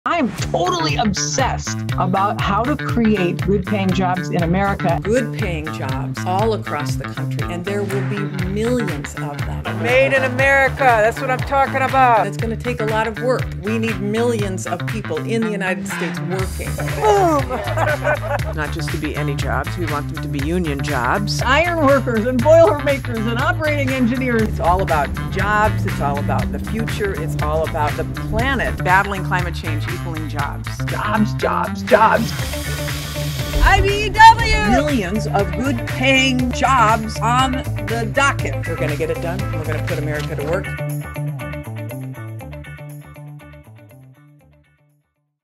Secretary Granholm's Labor Day Message
On Labor Day and every day, the Biden Administration is committed to creating millions of good-paying union jobs in every pocket of our country.

That’s what President Biden's Build Back Better agenda is all about: combating the climate crisis and JOBS, JOBS, JOBS.